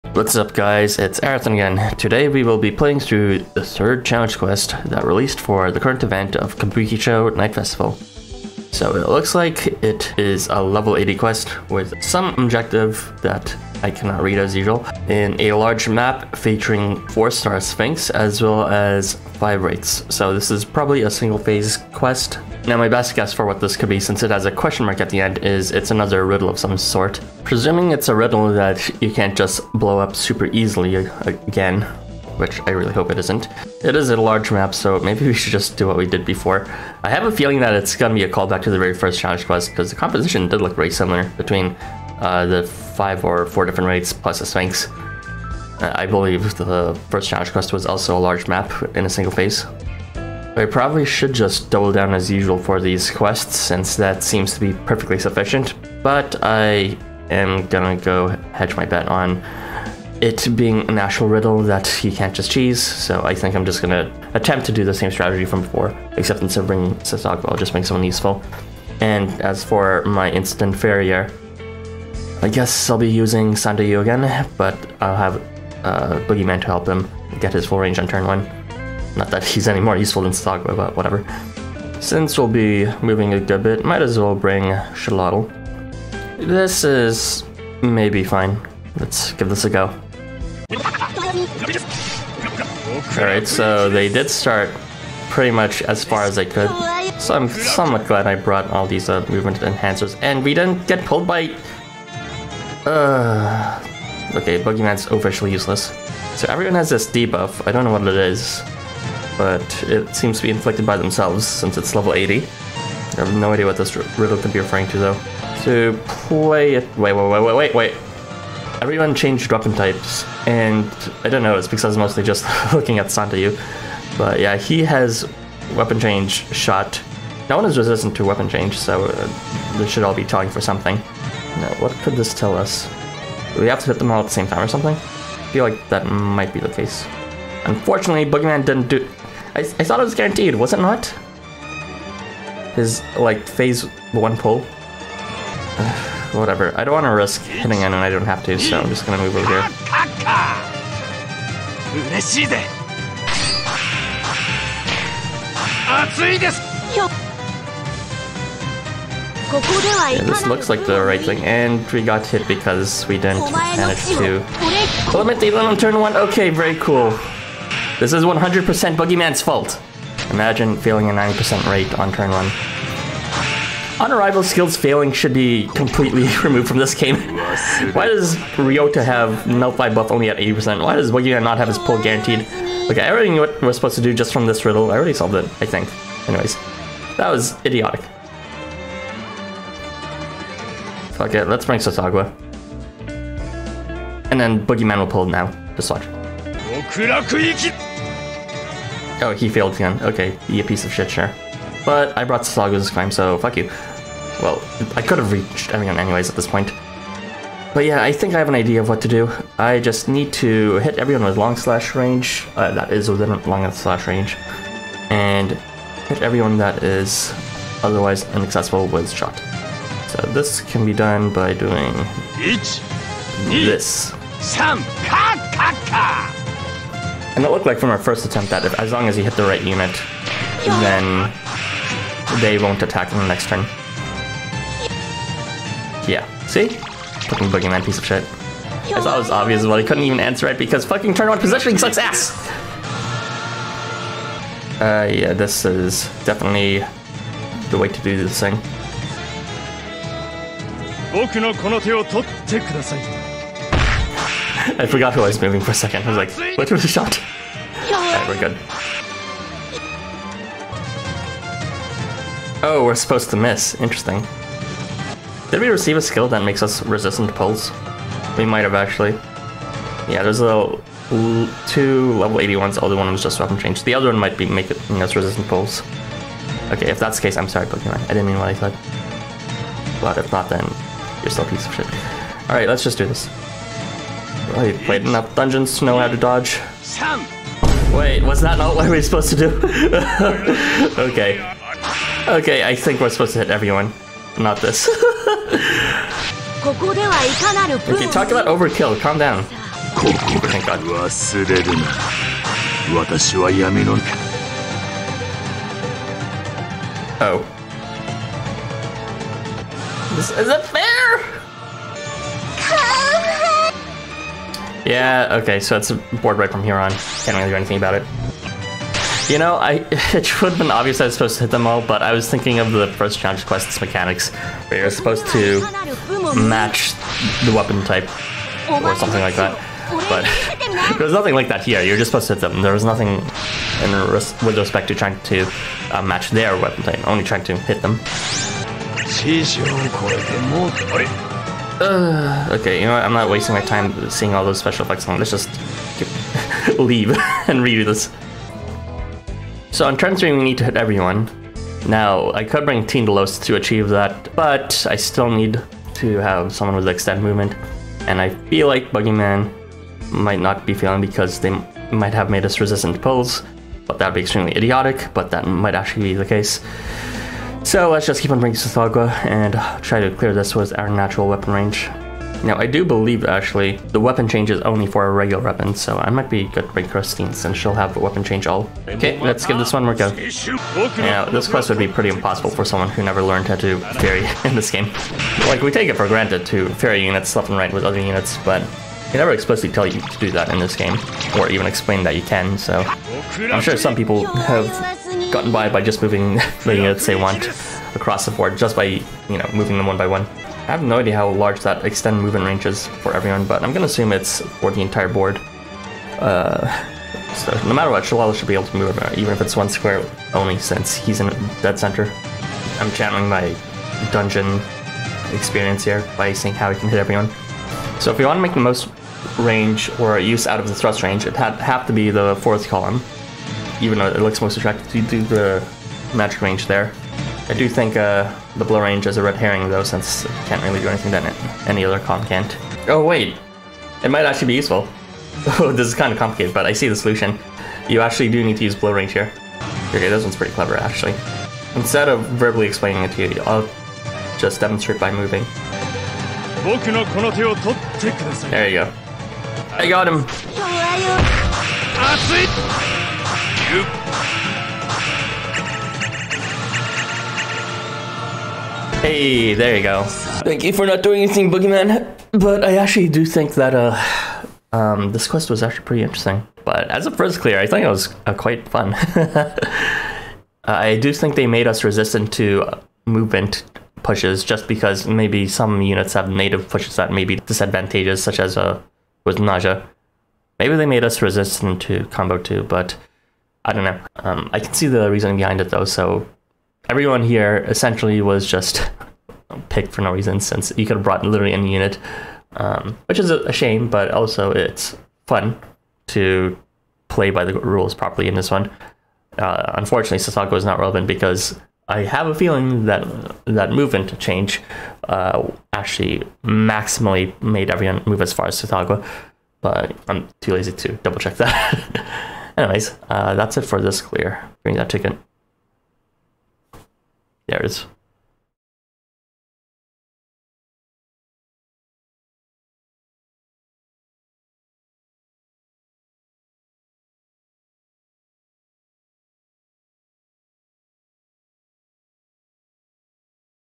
this easy quest beat me twice 😔 [Night Festival]
Quest Name:  [高難易度]スフィンクスの挑戦状

Each enemy charge attacks every turn unless they are hit on your preceding turn. On charge attack, they deplete a massiva amount of HP and remove all buffs from your team. They are vulnerable to Darkness, but are resistant to all other disabling statuses. The enemy bestows various skills to your team on arrival and every turn. On arrival, your team will be able to move anywhere on the board, and will be resistant to all movement-disabling statuses, but will lose all their CP every move phase end. On turns 1-3, your main members will change weapon ranges (allies normally resistant to weapon change will not ignore these), and will become resistant to every other weapon change. On turn 4 onwards, these allies will switch to None range. Members arriving from sub slots will instead immediately keep None range along with the other acquired skill effects.

0:00 Start! | Read video description for full quest details.
0:26 Team building 1 (blind)
1:30 Attempt 1 (blind)
3:10 Team building 2
3:15 Attempt 2
5:48 Team building 3
6:20 Attempt 3
7:38 Final thoughts | Phases: 1 | Elimination
8:39 End cards

Event Name: Kabukicho Night Festival (歌舞伎町ナイトフェスティバル)